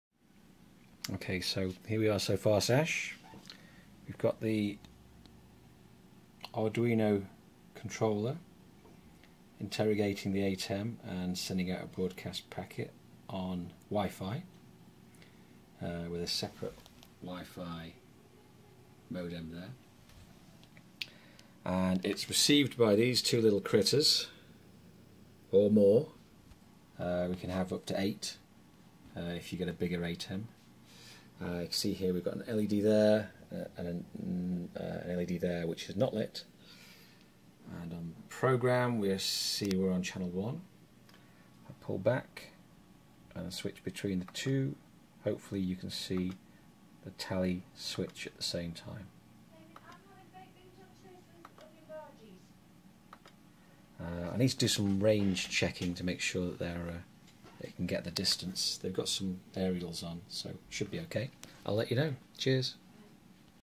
ATEM Tally project
I built a wireless tally solution for my good friend Sash using an Arduino Ethernet, Skaarhoj's library for ATEM switchers, and some Spark Cores to wirelessly transmit tally status to cameras.